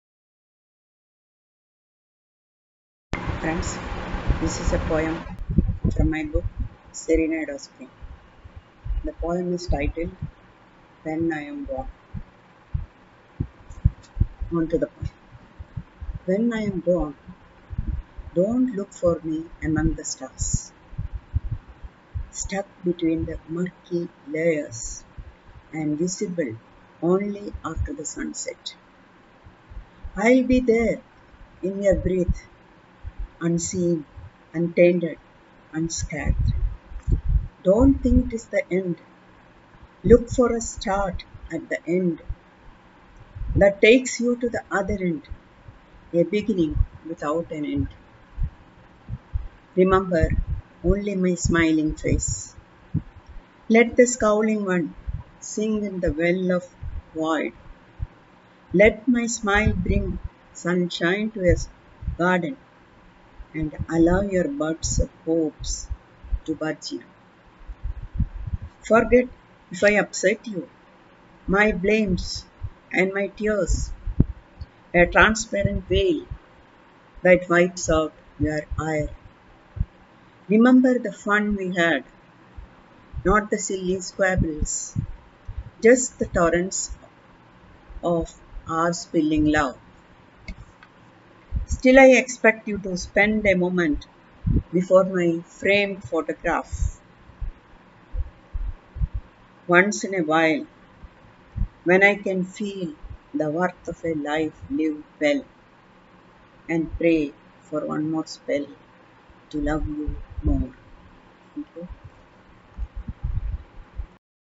Senior Citizen Contest-2021 | English Poetry | Pankajam Kottarath(67 Years)
Pankajam Kottarath, a retired Dy Manager/Finance from BHEL, a bilingual poet, fiction writer and  reviewer and have won many awards.